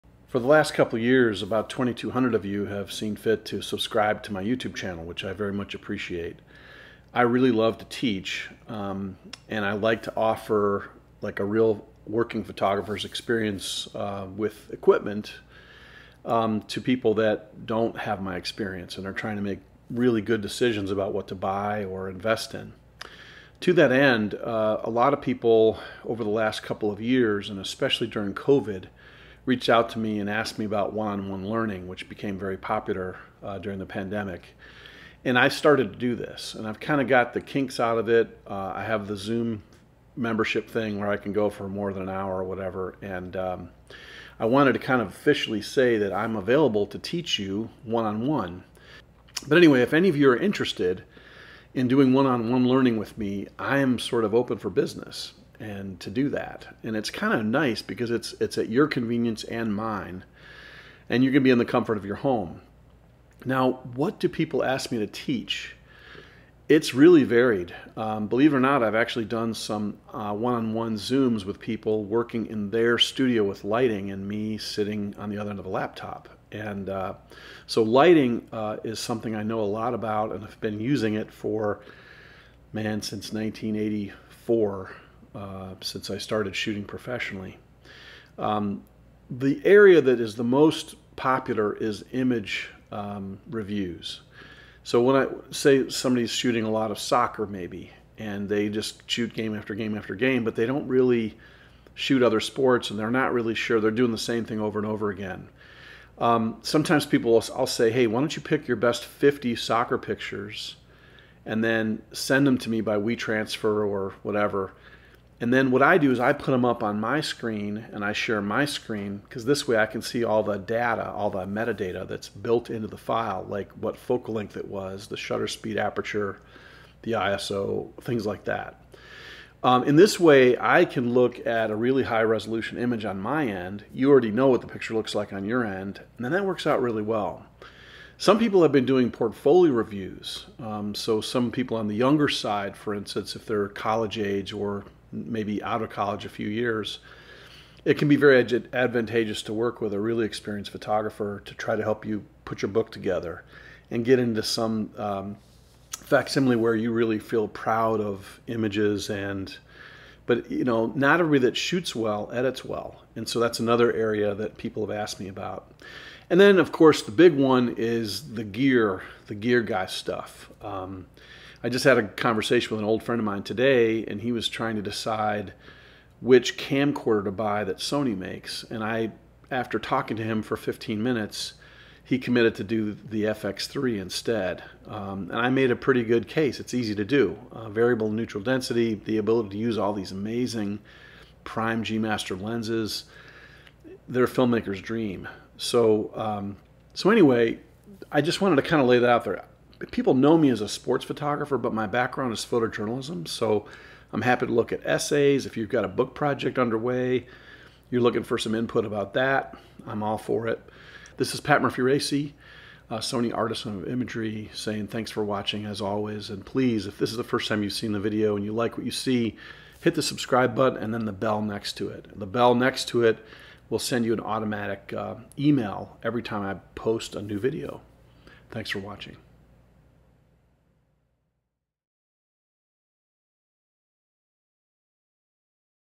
One on One learning Video with Sony Artisan Patrick Murphy-Racey
For the past year or so during the pandemic, I've been offering One on One learning through Zoom to photographers that are seeking to learn new things, expand their understanding of lighting, and especially by doing image review sessions where we pick apart the elements that can come together to make a great photograph.  If you have been enjoying my YouTube videos, perhaps you would like to try a One on One session to improve some aspect of your work.  You can find me at . We can find a time that is convenient for both you and me and get started.